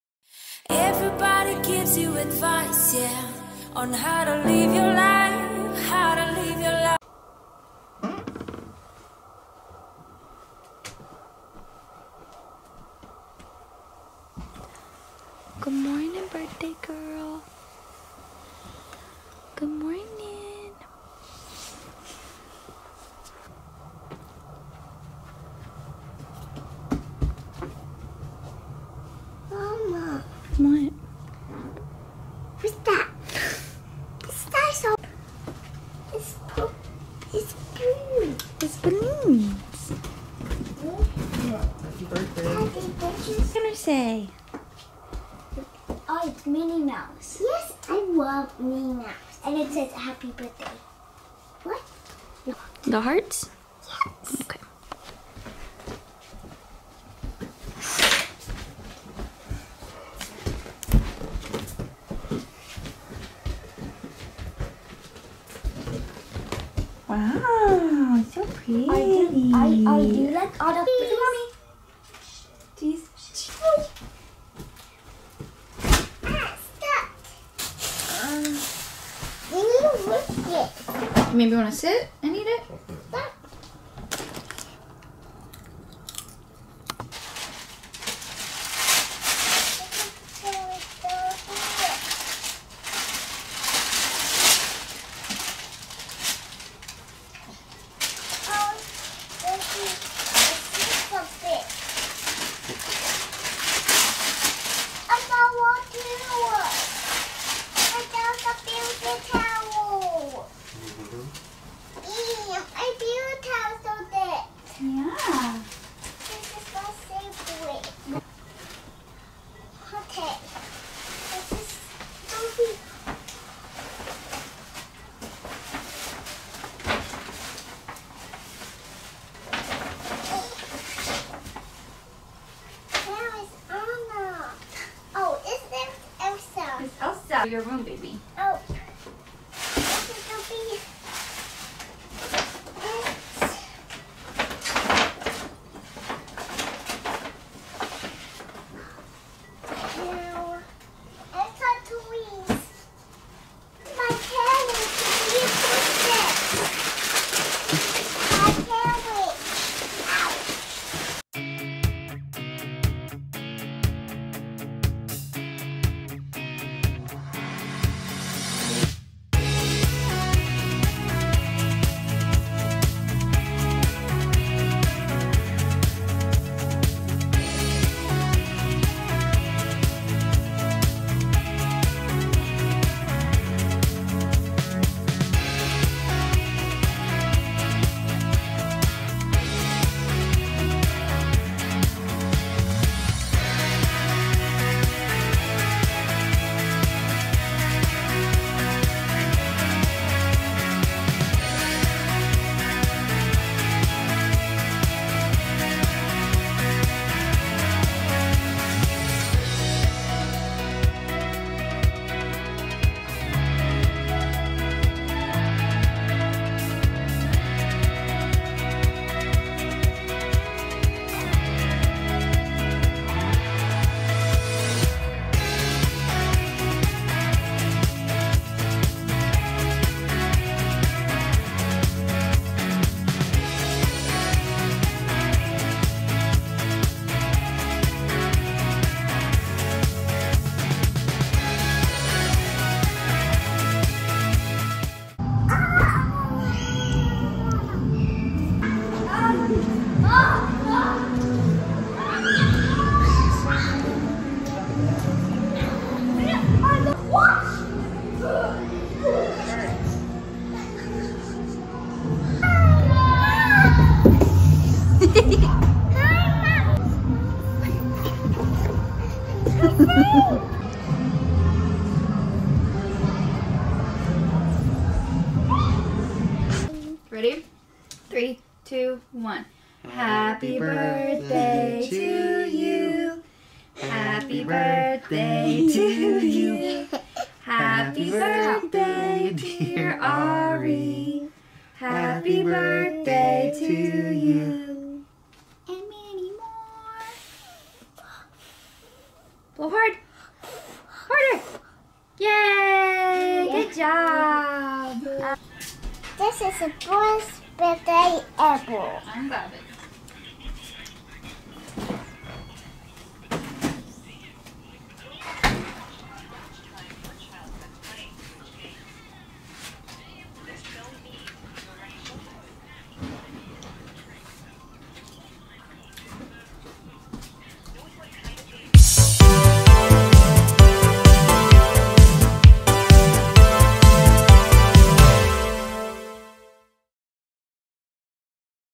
Ahri Turns 3 Years Old || Birthday Vlog
Ahri turns 3 years old || Birthday

Amazon Must Haves
My Workbook I made for kids
My wishlist